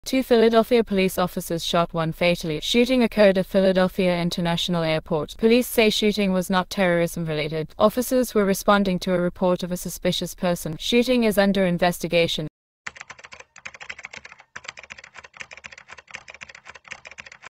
2 Philadelphia police officers shot 1 fatally at airport garage
or